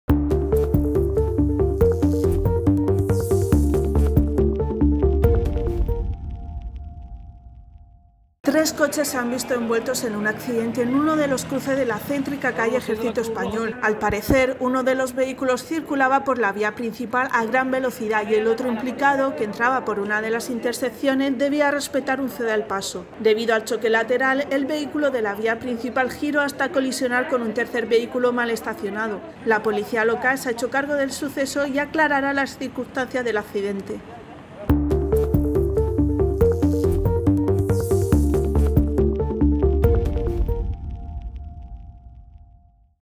Accidente múltiple en la calle Ejército Español
Tres coches se han visto envueltos en un accidente en uno de los cruces de la céntrica calle Ejército Español. Al parecer uno de los vehículos circulaba por la vía principal a gran velocidad y el otro implicado, que entraba por una de las intersecciones, debía respetar un ceda el paso. Debido al choque lateral, el vehículo de la vía principal giró hasta colisionar con un tercer vehículo mal estacionado. La Policía Local se ha hecho cargo de suceso y aclarará las circunstancias del accidente.

Redacción y locución: Alicia Martínez